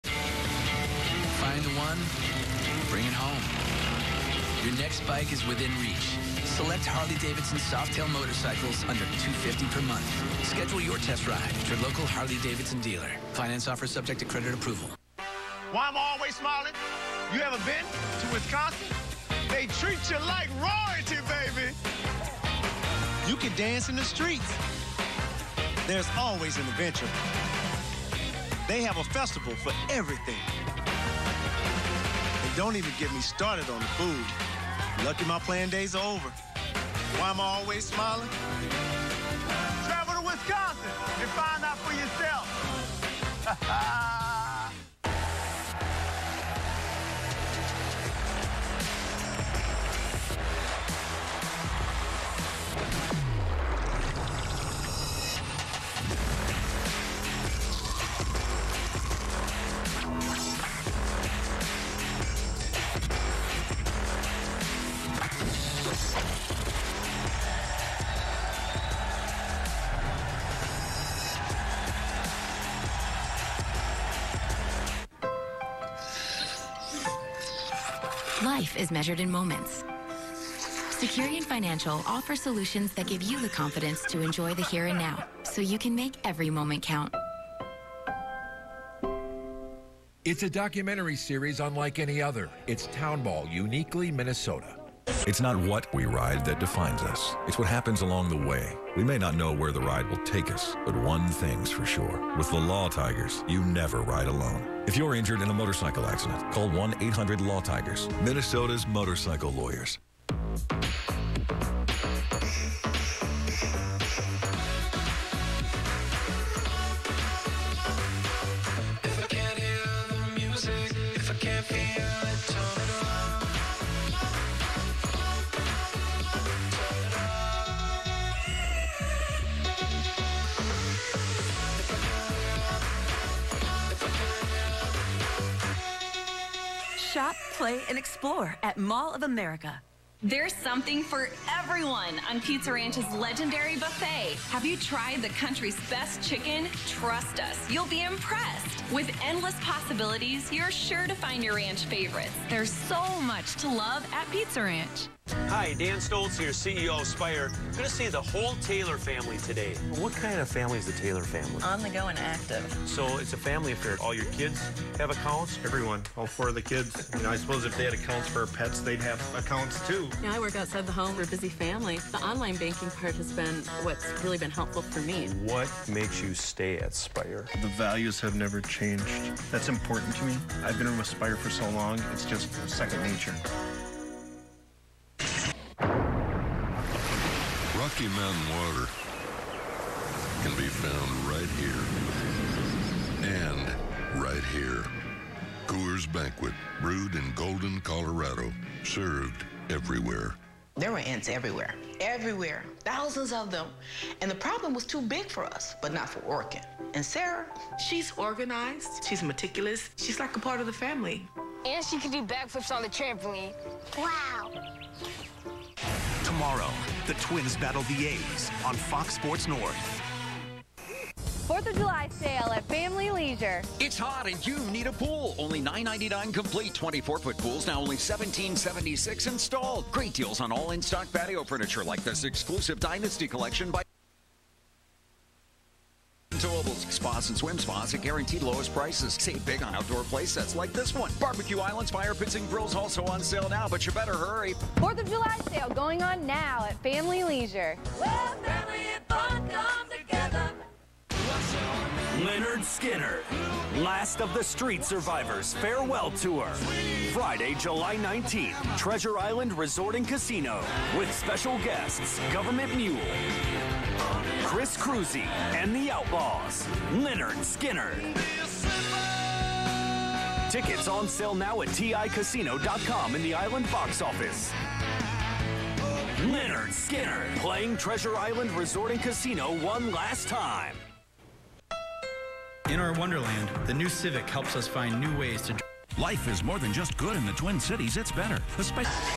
Fox Sports North - July 2, 2019 Commercials (S-VHS)
Self-recorded
Description: During an MLB game Twins vs. Athletics. Fox Sports North; July 2, 2019